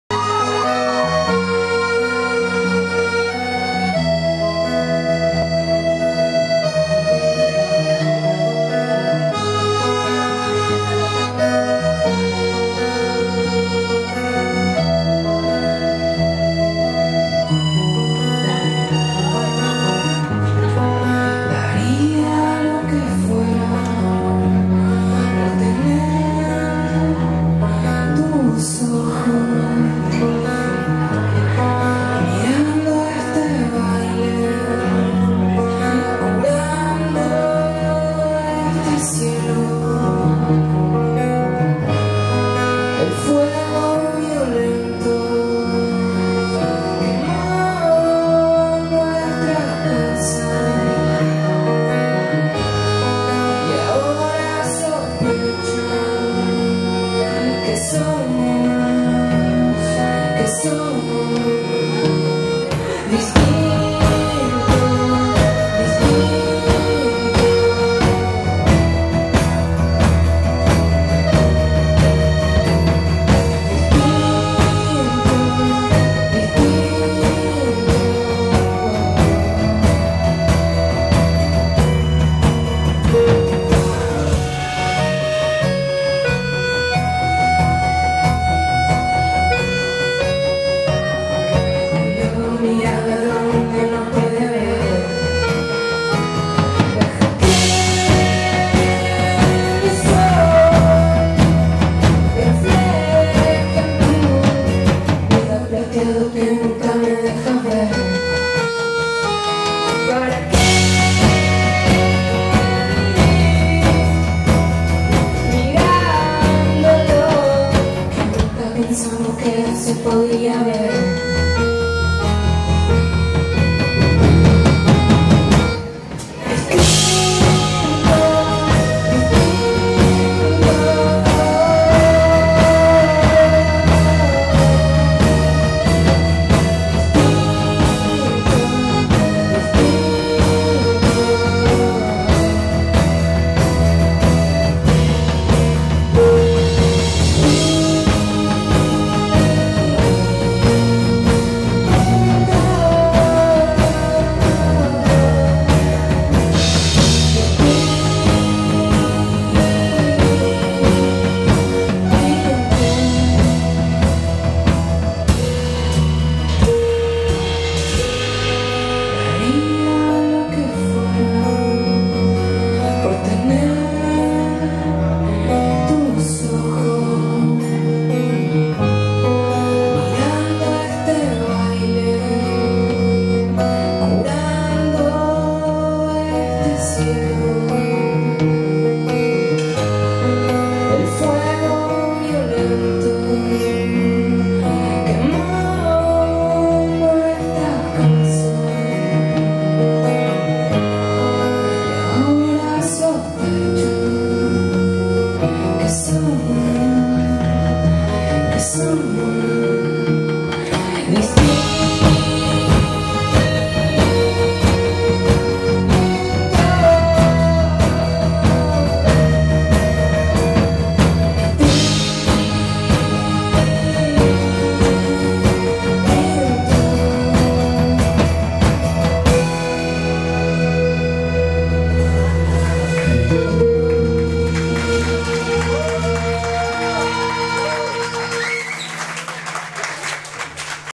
Prehistoricos. Bar Loreto
viernes 29 de marzo en Bar Loreto, show completo de Prehistoricos!

Dj Haiti en la Fiesta

Loreto 435/Bellavista